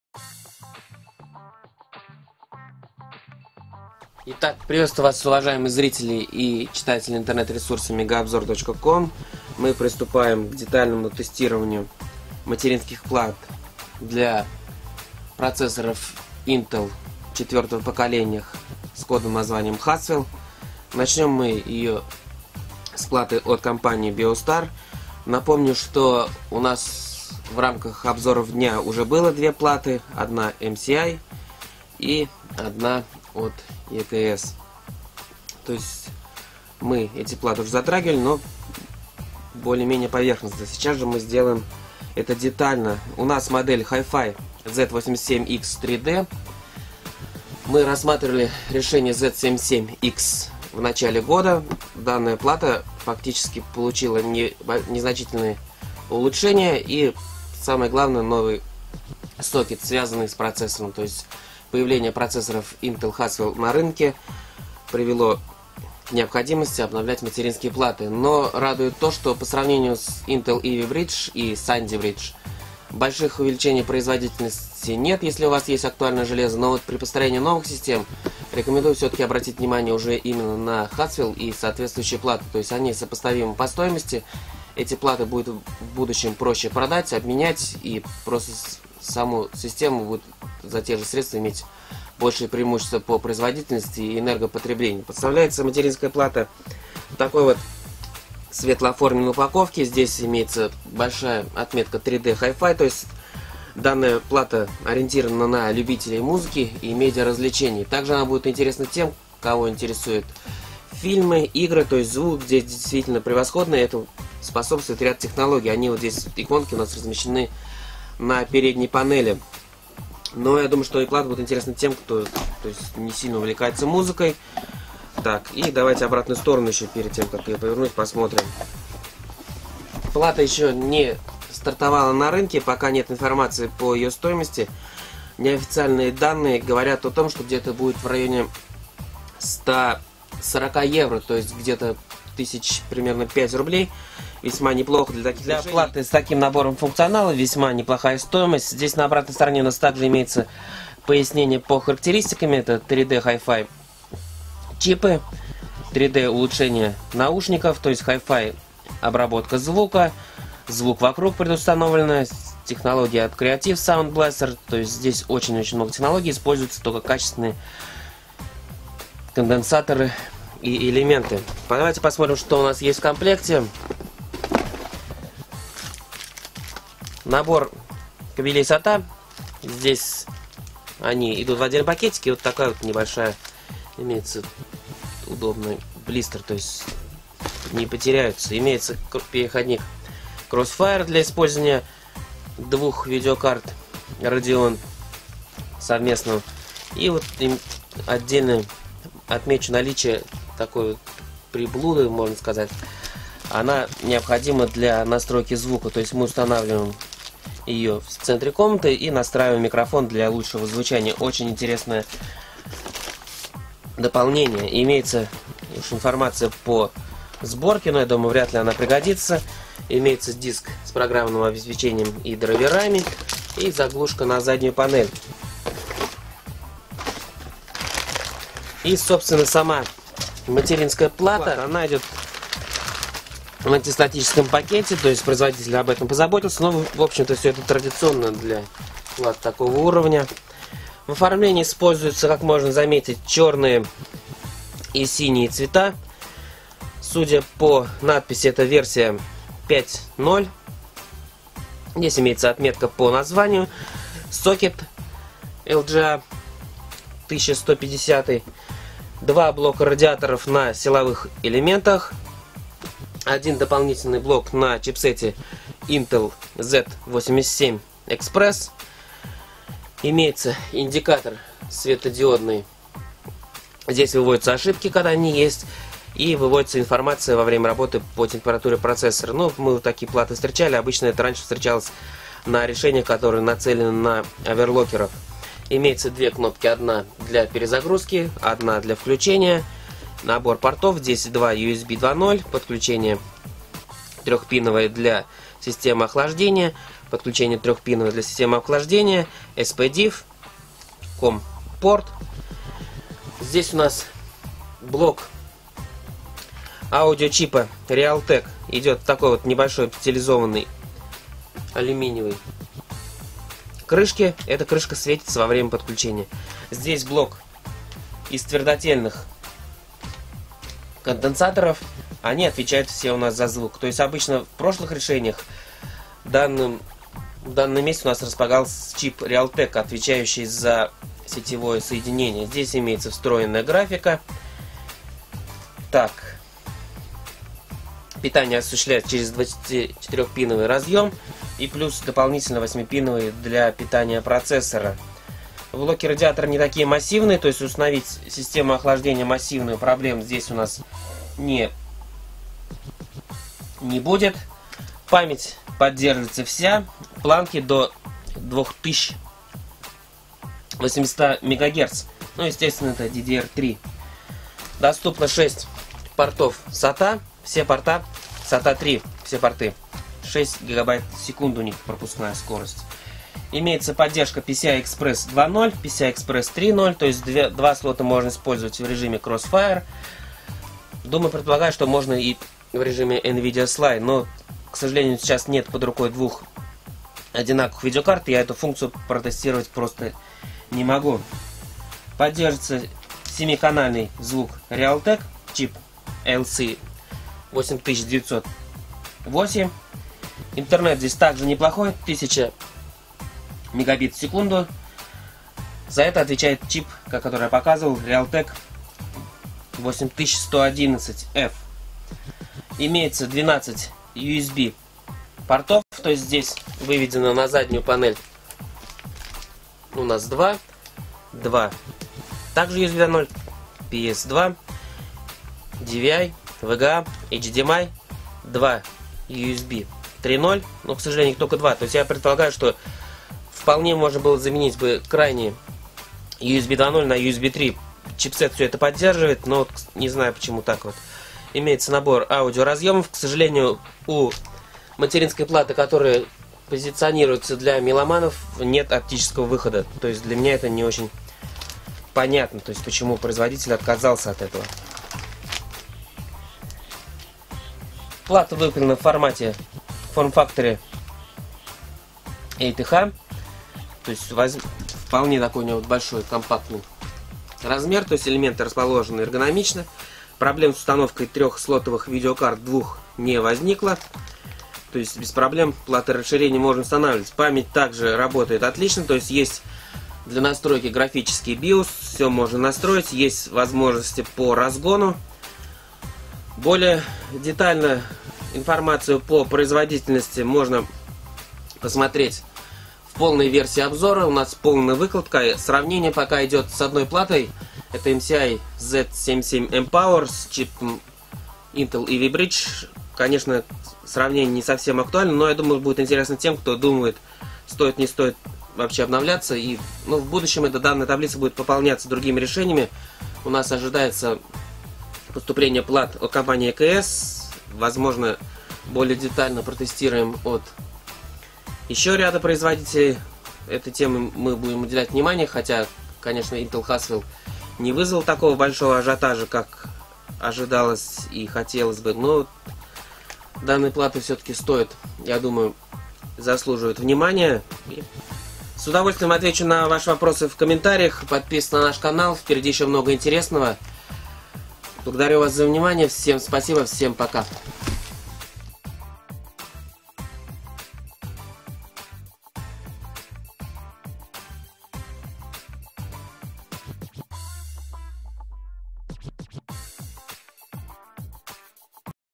Обзор Biostar Hi-Fi Z87X 3D
Biostar Hi-Fi Z87X 3D продемонстрировала стабильную работу во время тестирования, проблем и сбоев зафиксировано не было.